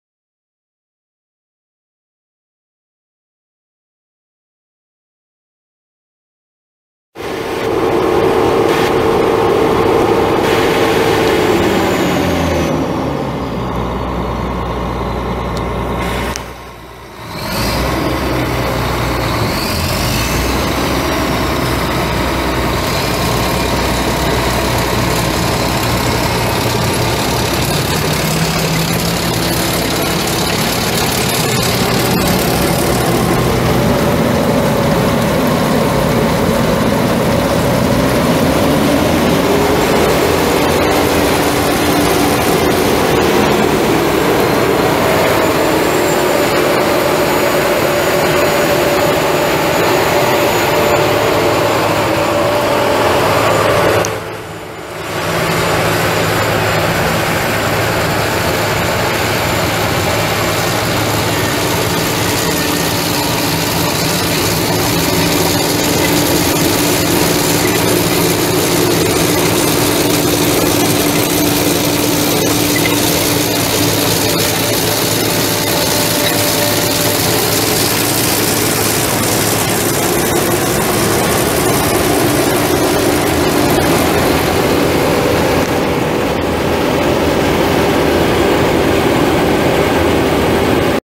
2002 Lexion 480R combine for sale | no-reserve Internet auction August 23, 2017
2002 Lexion 480R combine
3,635 engine hours on meter
Caterpillar C12 12.0L turbo diesel engine
Hydrostatic transmission
Feederhouse
Lateral tilt
Multi-speed
Reverser
AM/FM
Power mirrors
Air suspension seat
Instructor's seat
Cebis computer system with printer
Twin rotor
Three speed
Chaff spreader
Straw chopper
Power folding bin
Bin extensions
Work lights
Operator and service parts manuals
20.8R42 dual front tires
420/85R28 rear tires
Serial 86600816
It's located in Brownell, KS, and we're selling it to the highest bidder regardless of price.